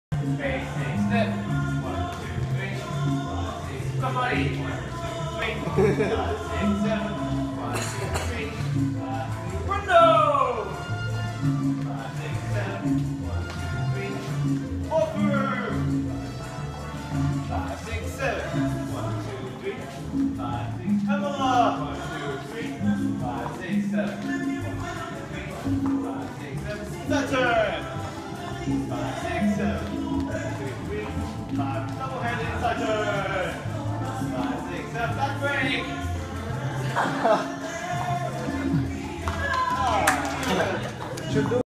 TSF Sunday Practice - 2 Guys For Every Girl
How we overcome the lack of girls at Sunday Salsa Practice.